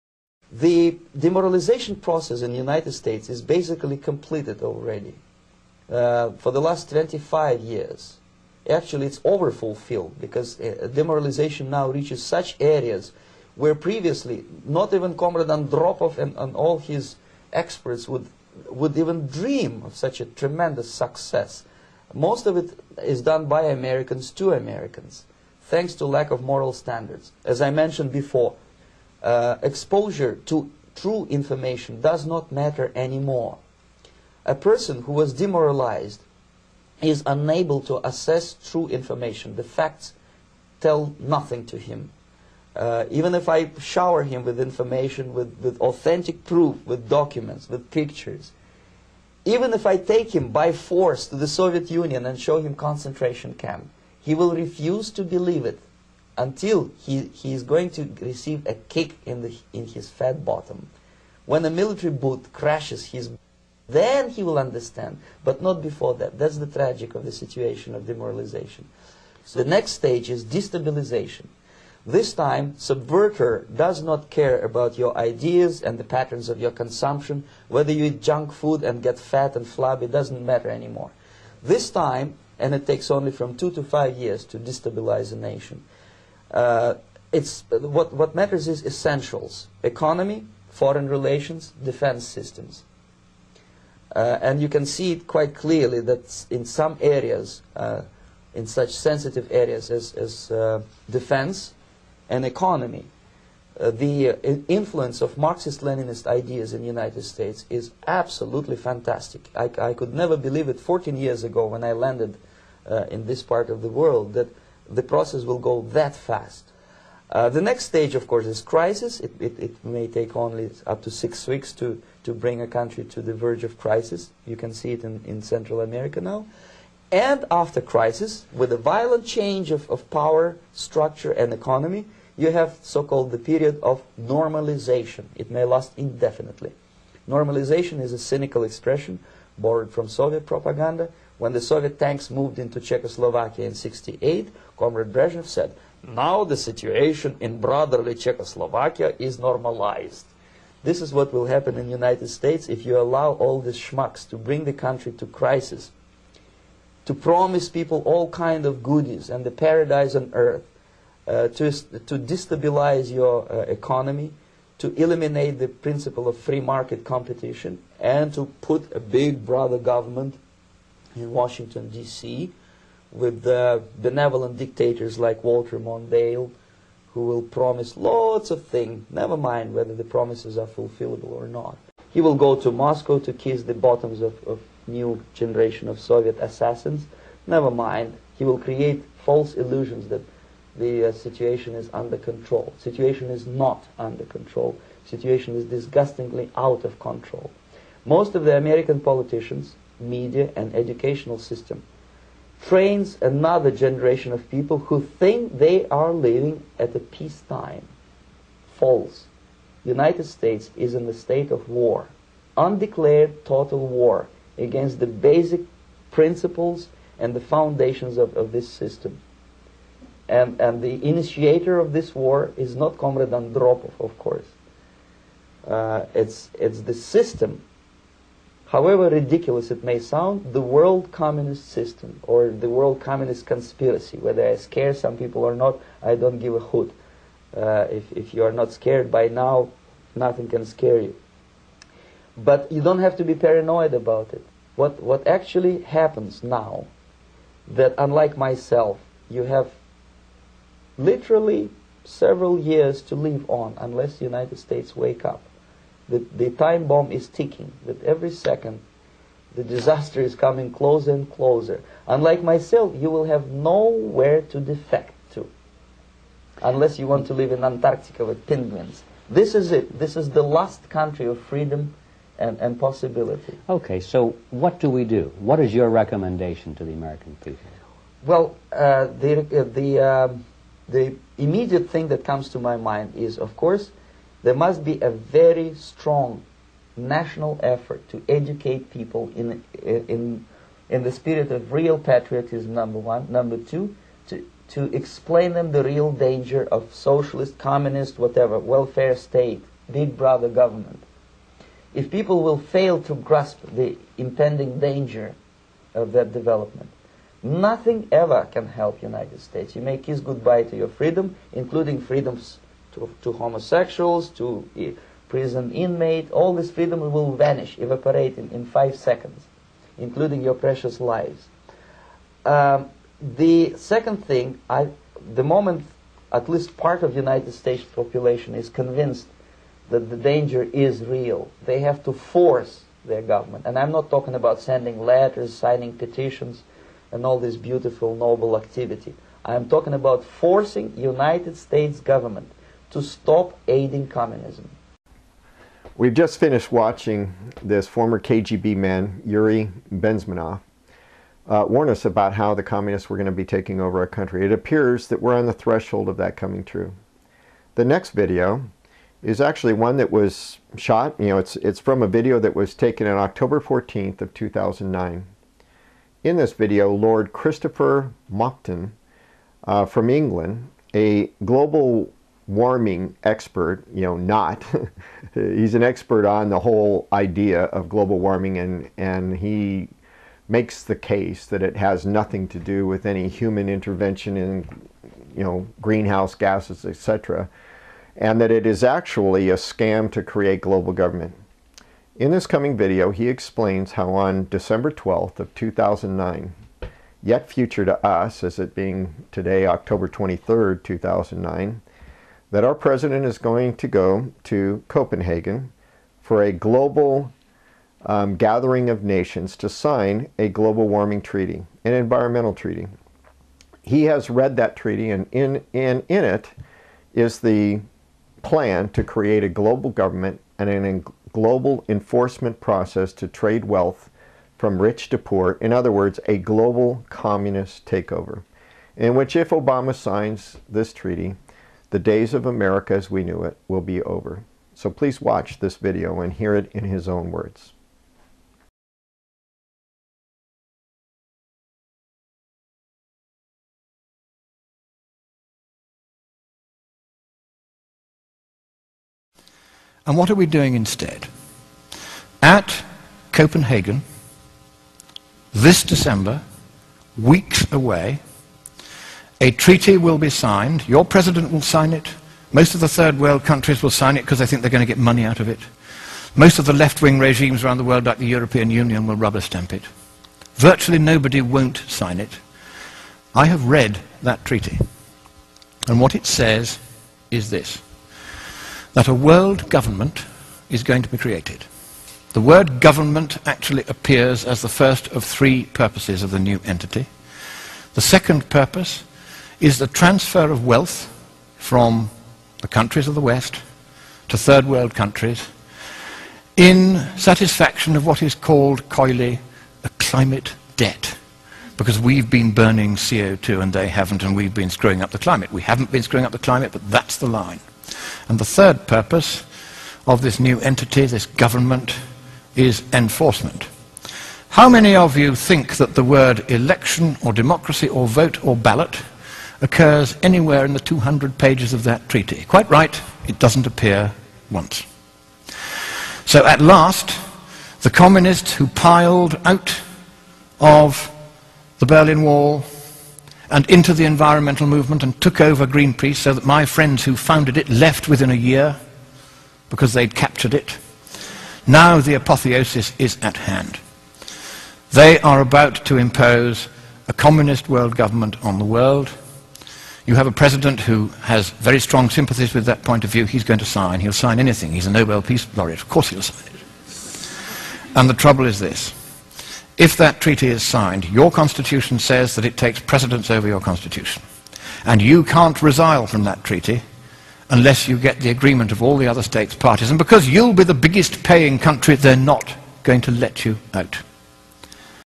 Part 3 of 7 Current Events October 23, 2009
A Biblical Perspective of Current Events as of October 23, 2009. There are many things happening around the world that not coincidently are lining up with Biblical prophecy. Hopefully this video series will help bring some light to our current events.